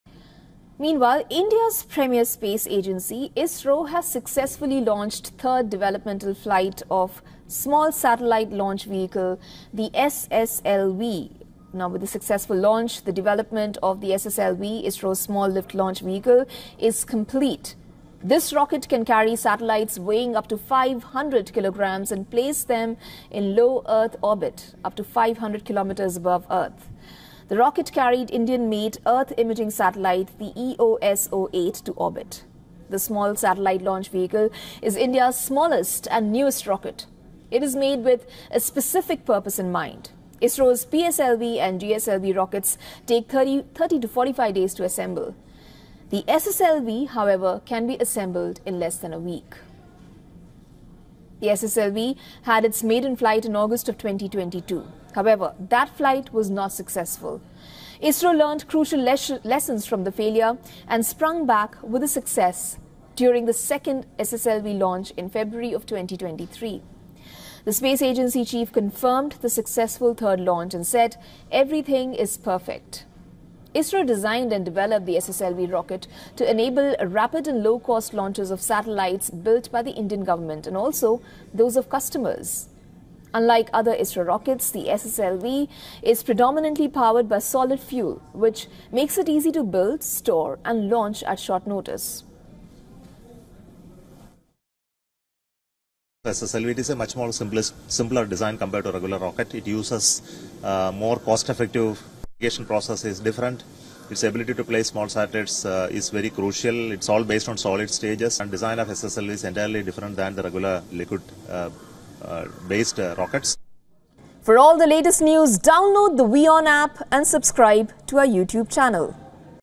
SSLV is meant for launching satellites weighing less than 500 kg | Latest News | WION
The SSLV-D3 in its third and final development flight lifted off from the first launch pad of the Satish Dhawan Space Centre at 9.17 am.